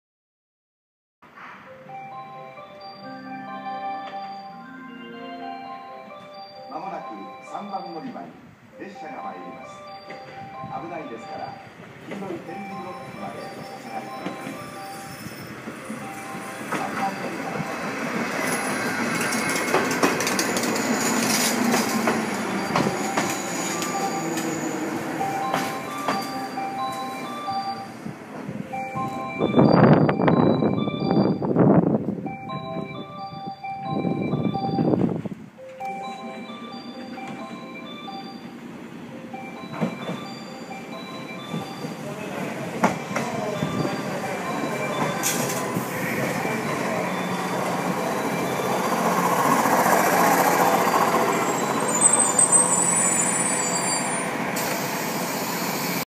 紀勢本線 105系 普通列車と381系スーパーくろしお21号
串本駅で、スーパーくろしお21号を待っているときに撮りました。